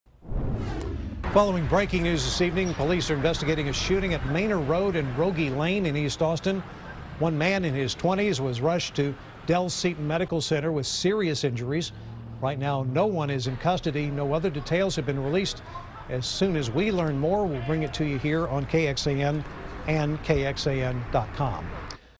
Man in his 20s seriously wounded in east side shooting
Medics were called to the intersection of Rogge Lane and Manor Road at 8:34 p.m. for the report of a gunshot wound.